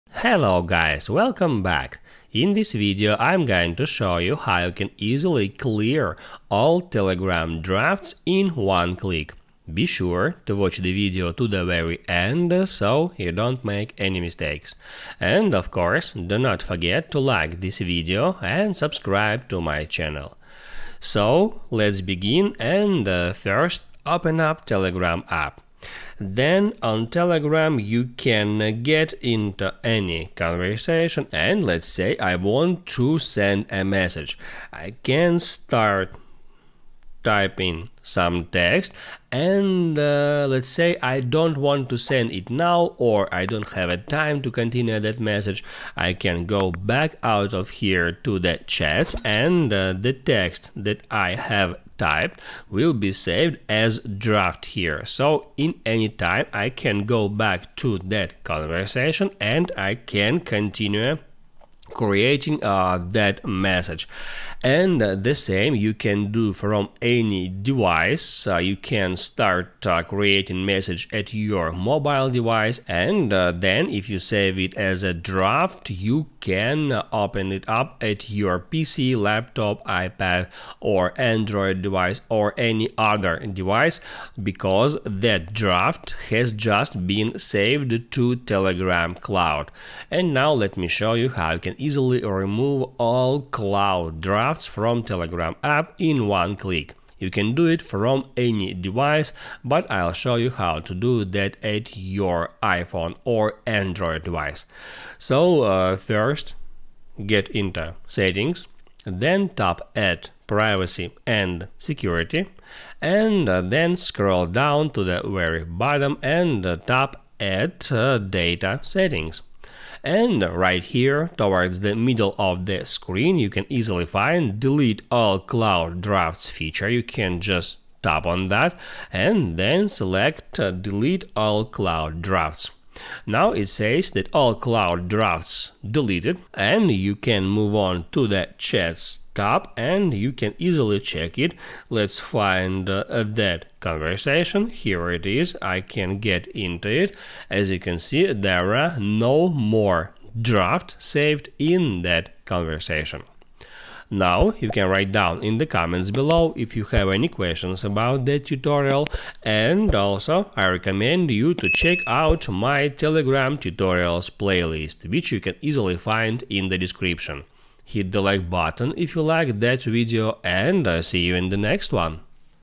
How To Clear All Drafts on Telegram in One Click From Any Device
If you are looking for a video about how to clear drafts on telegram, here it is!